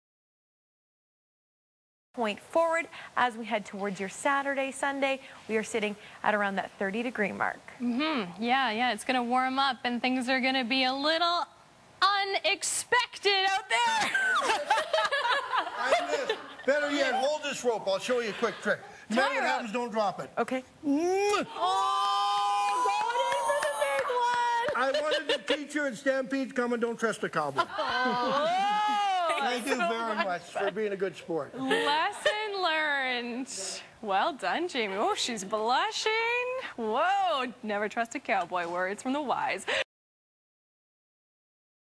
A Calgary Stampede Lesson
Bud Edgar joking around with Calgary CTV weather girl Jamie Mauracher.